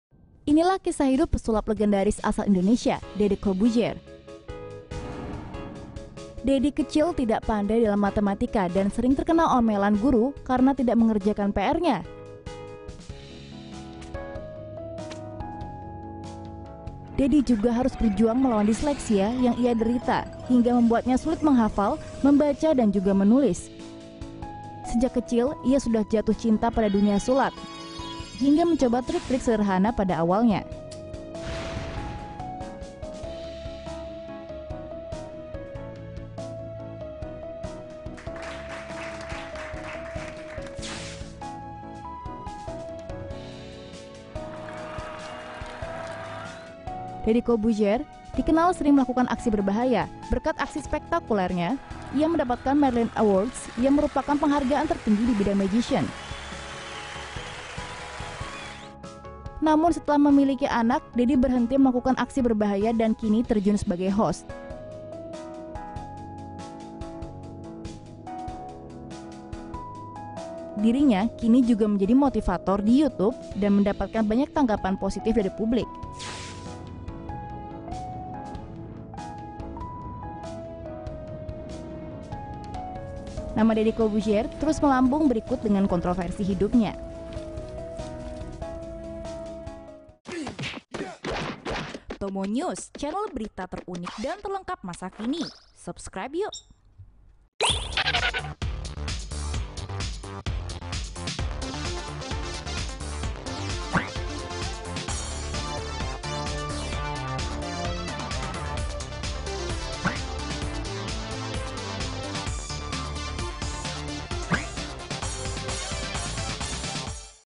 Kisah Sukses Mereka: Episode 1 - Deddy Corbuzier - TomoNews
Berikut beberapa kisah inspiratif beberapa tokoh hingga mereka mencapai sukses